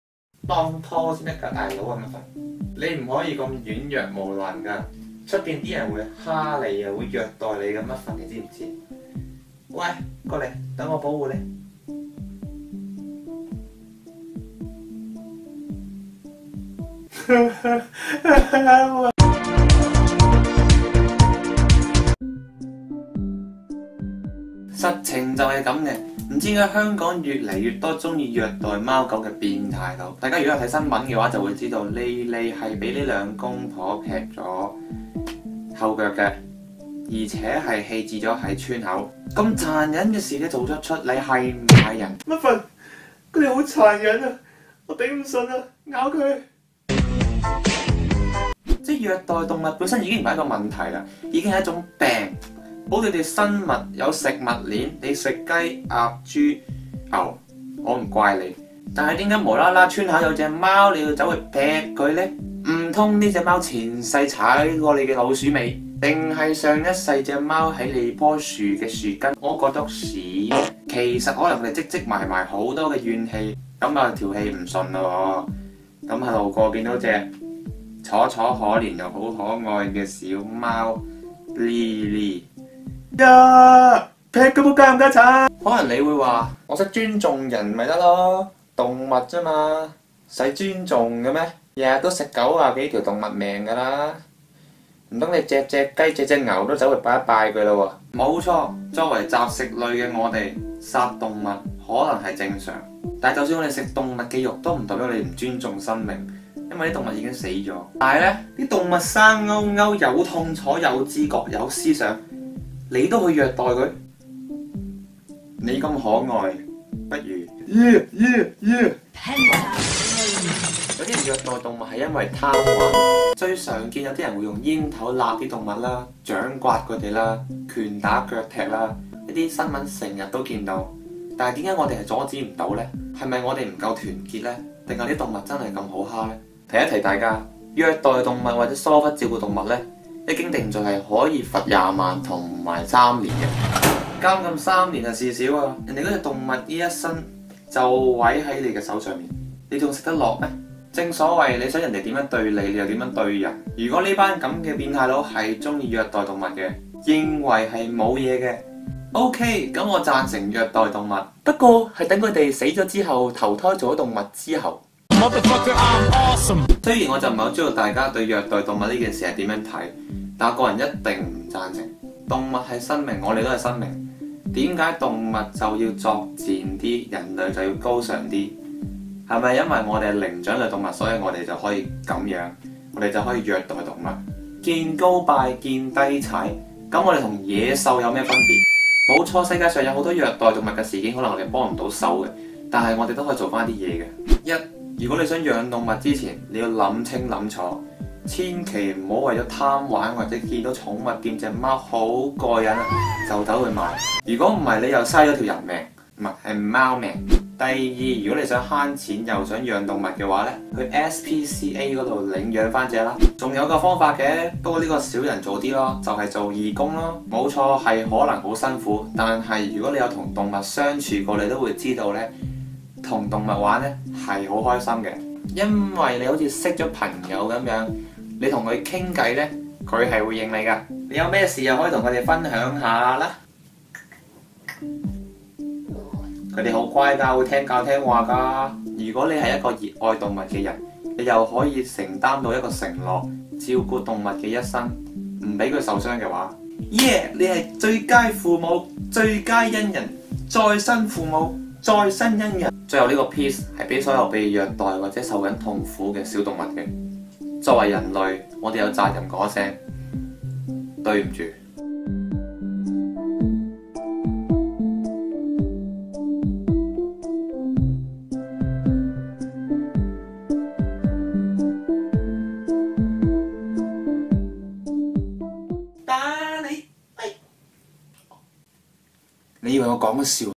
【小貓麗麗 不屈虐貓狂徒盧隸獸！】動物受虐 人人有責﹖
麗麗被砍掉後腿也可以堅強生存，
為何作為人類的我們不出一分力，
阻止虐待動物的慘劇再次重演呢?

星島日報「麗麗」可坐下仍吊鹽水 斬貓腳案婦人被捕夫自首